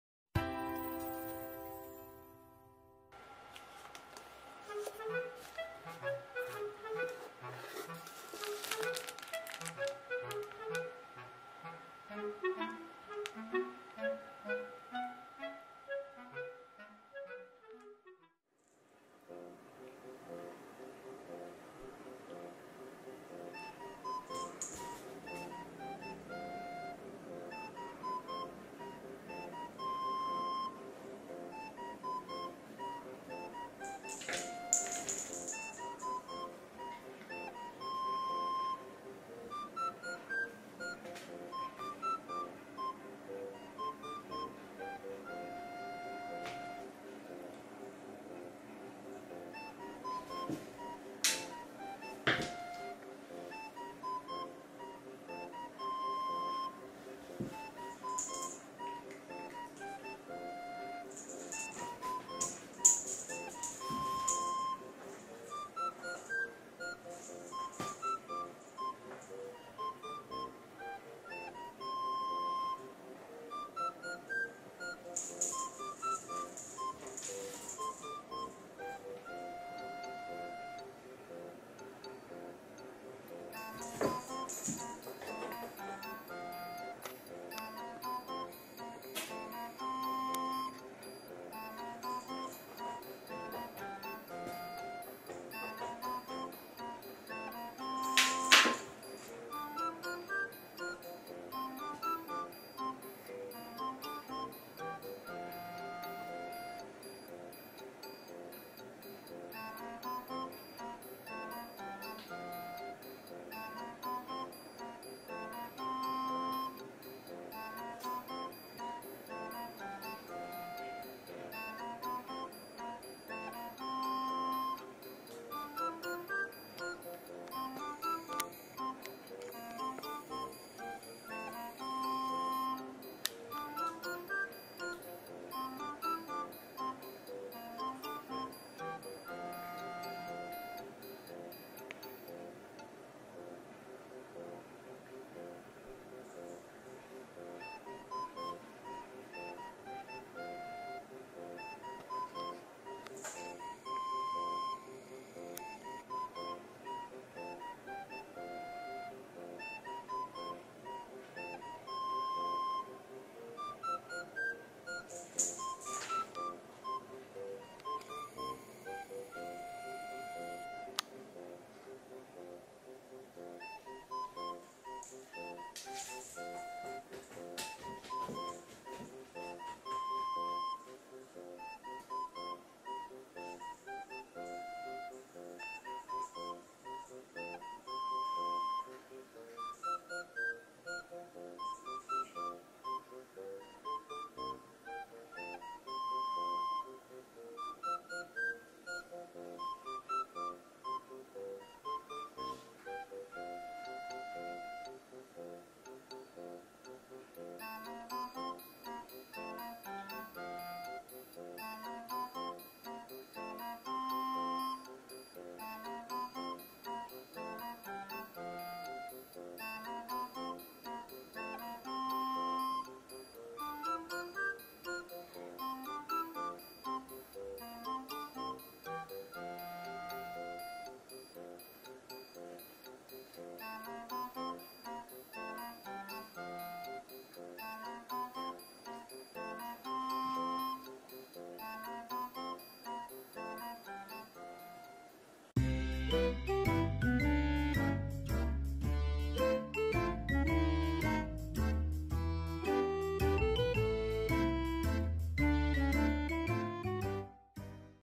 おまけでもらったマタタビボールで楽しそうに遊ぶ猫ズ。
茶トラ猫の「ちゃい」と、サバトラ猫の「すし」のドライフードを購入したら、おまけのおもちゃを頂きました。マタタビと鈴が入ったマシュマロボールという商品らしいです。ボールで遊んだり、マタタビに興奮して自分の尻尾で遊んだりする姿が可愛い猫の動画をご覧下さい。